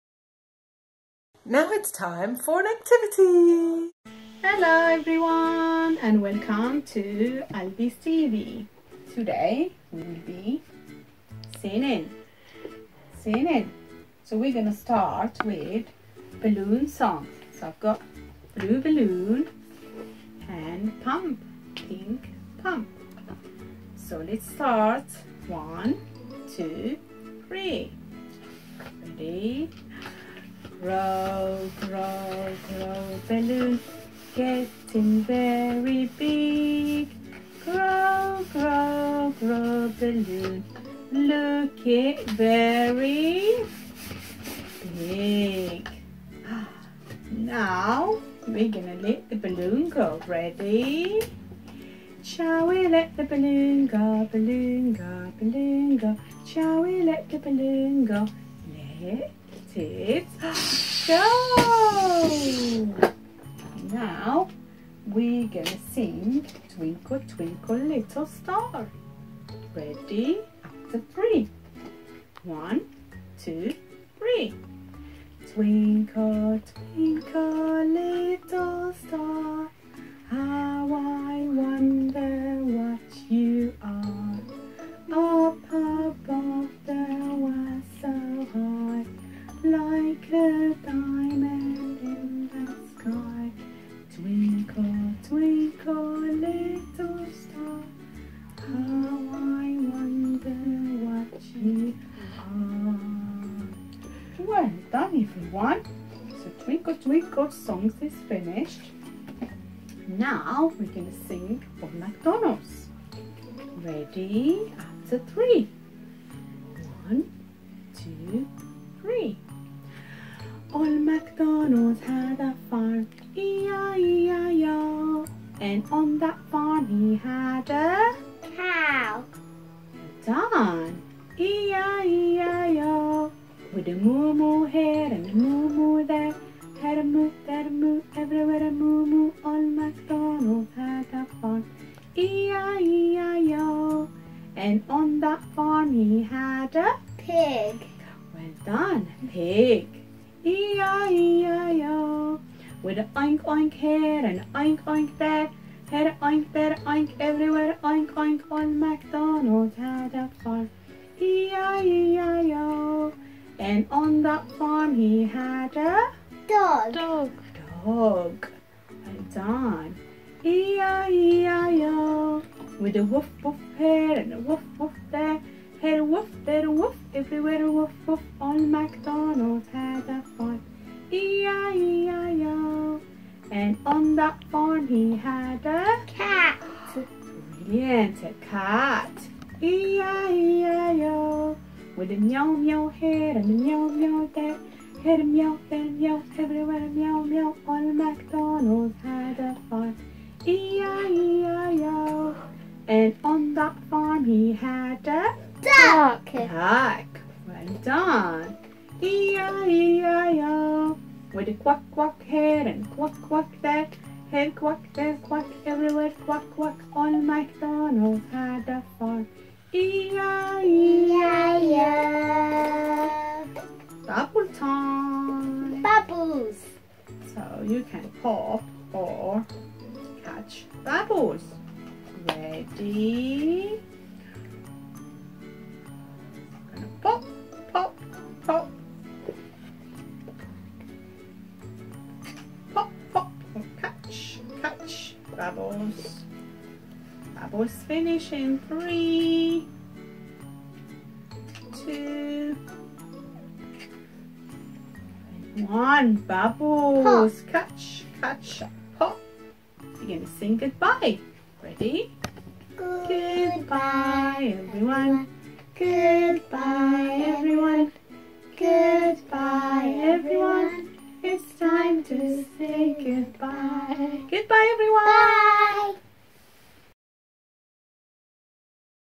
AlbyTV: Balloon and Bubbles Activity Time
Happy Monday! Get ready for the week with balloon and bubble time with Hanane!